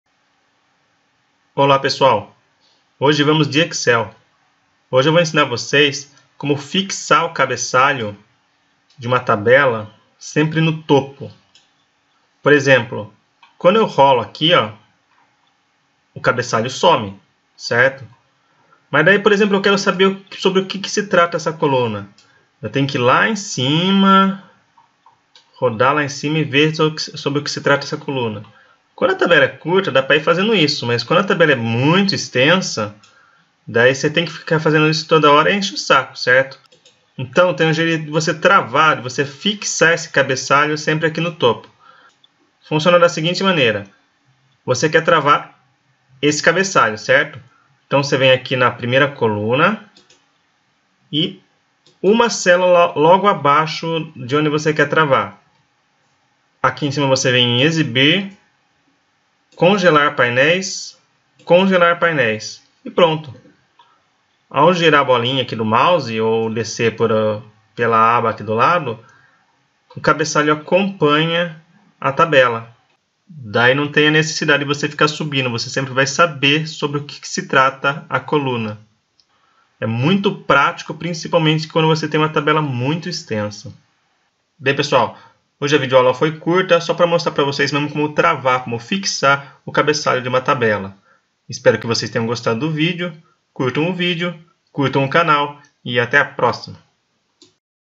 Fixar o cabeçalho de tabelas - Excel
Aprenda a congelar o cabeçalho de uma tabela para ela ficar sempre visível. Ideal para planilhas muito extensas. Tudo através da opção "congelar paineis".